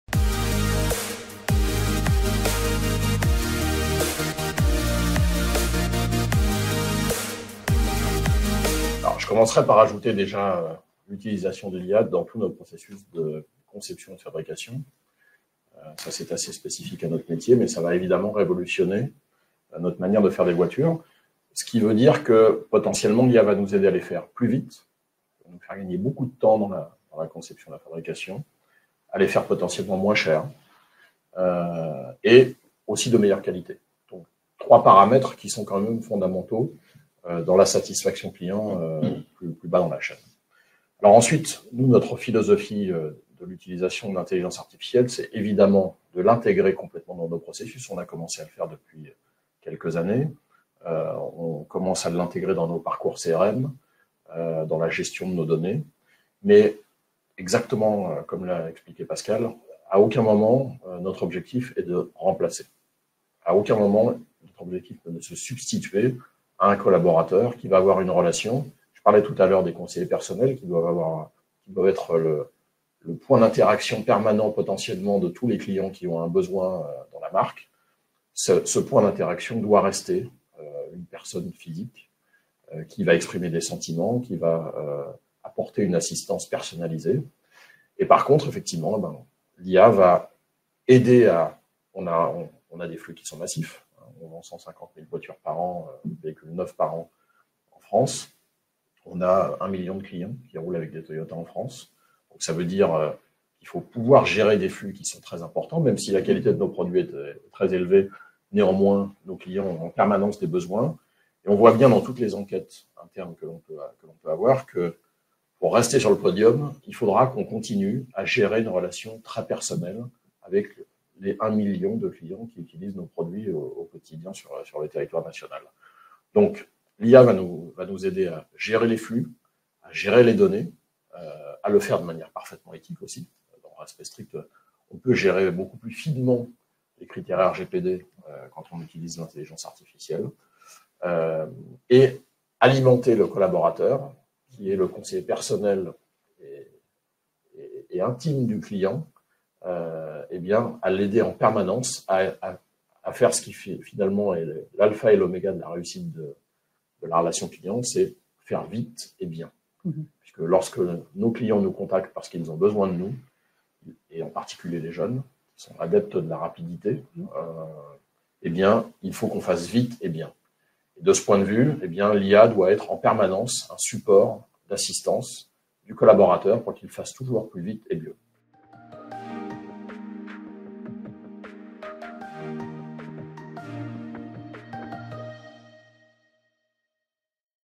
Toyota : l'IA au service de la conception automobile et du CRM (Insights Ignition 03 2024)
par Franck Marotte, Président & CEO, Toyota France.